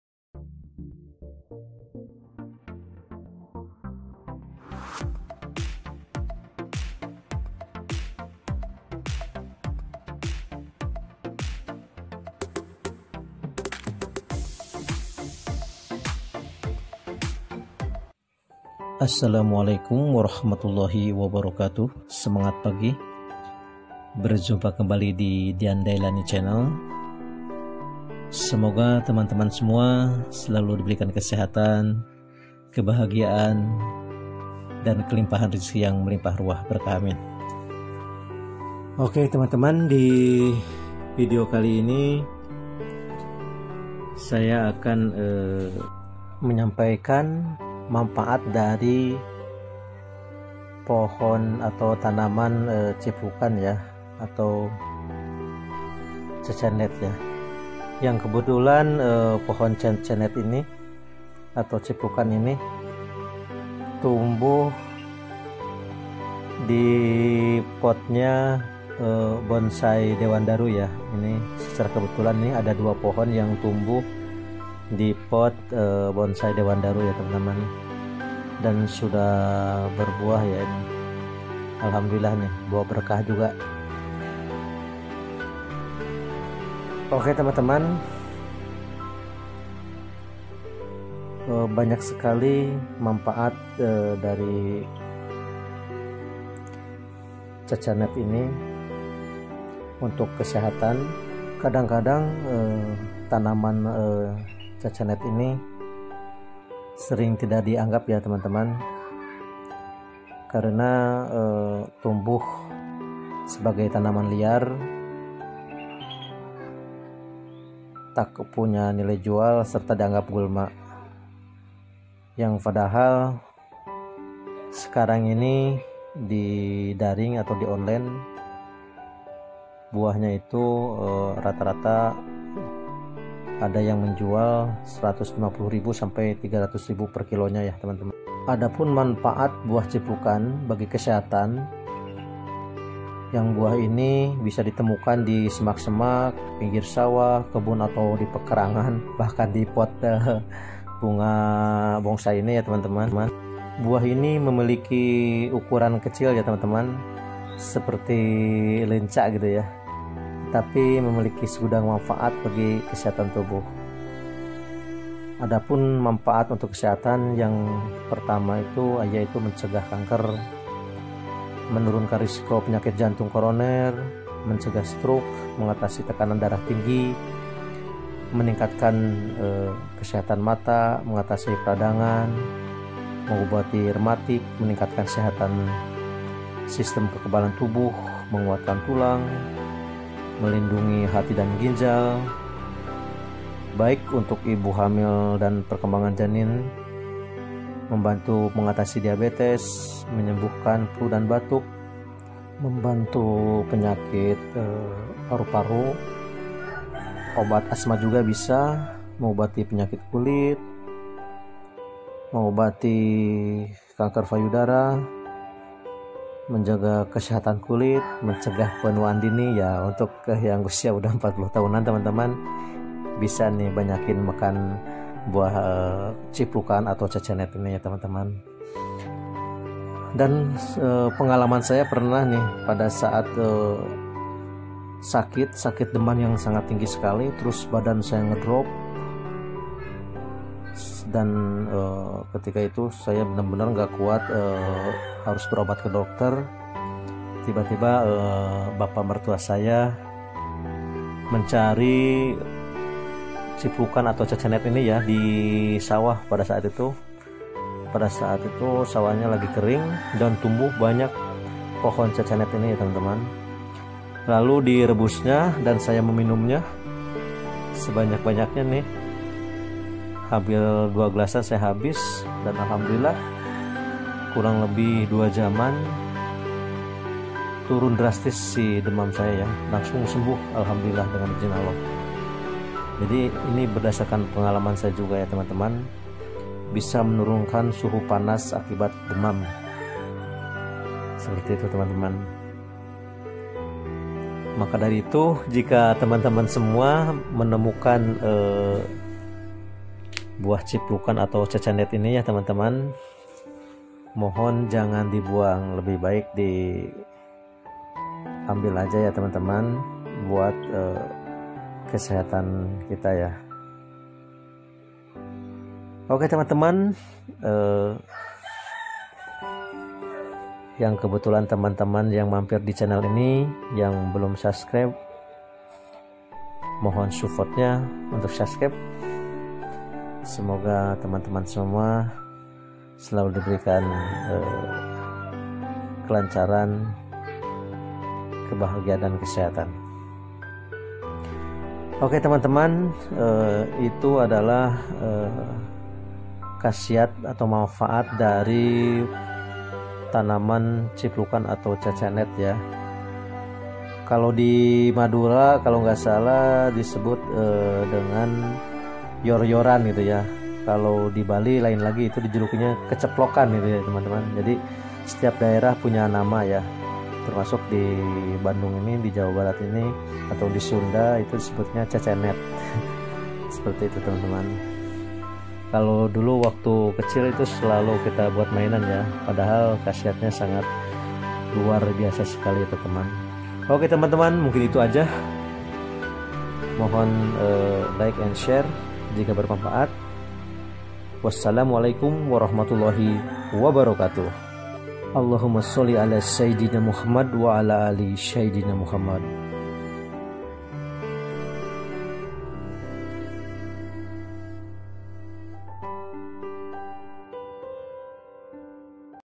KHASIAT CIPLUKAN/CECENET UNTUK KESEHATAN
Khasiat Ciplukan/Cecenet untuk Kesehatan Bisa Jadi Obat .Sering Tak Dianggap, Ternyata Daun Ciplukan Bisa Jadi Obat Buah ini rasanya kecut-manis, berwarna hijau-kuning, dan dibungkus kulit tipis. Buah ciplukan sempat dipandang sebelah mata dan dikenal sebagai tanaman liar, tak punya nilai jual, serta dianggap gulma.
Ciplukan memiliki nama latin Physalis angulata L. Tanaman ini banyak tumbuh di kebun, tegalan, tepi jalan, semak, atau hutan bahkan dalam pot bunga.Ciplukan memiliki nama yang berbeda-beda di setiap daerah. Buah tersebut terkenal dengan nama morel berry di Inggris, ceplukan di Jawa, cecenet di Sunda, keceplokan di Bali, dan leletokan di Minahasa.
manfaat buah ciplukan yang jarang diketahui:

Mencegah kanker
Menurunkan risiko penyakit jantung koroner
Mencegah stroke
Mengatasi tekanan darah tinggi
Meningkatkan kesehatan mata
Mengatasi peradangan
Mengobati rematik
Meningkatkan sistem kekebalan tubuh
Menguatkan tulang
Melindungi hati dan ginjal
Baik untuk ibu hamil dan perkembangan janin
Membantu mengatasi diabetes
Menyembuhkan flu dan batuk
Membantu penyakit paru-paru
Obat asma
Mengobati penyakit kulit
Menurunkan suhu panas akibat demam
Mengobati kanker payudara
Menjaga kesehatan kulit
Mencegah penuaan dini

Ciplukan sedang naik daun beberapa waktu terakhir. Buah ini banyak dijajakan di supermarket atau dijual secara daring (online).
Harga ciplukan segar per kilonya bervariasi. Mulai dari Rp 150.000 sampai Rp 300.000.

Salam Herbal,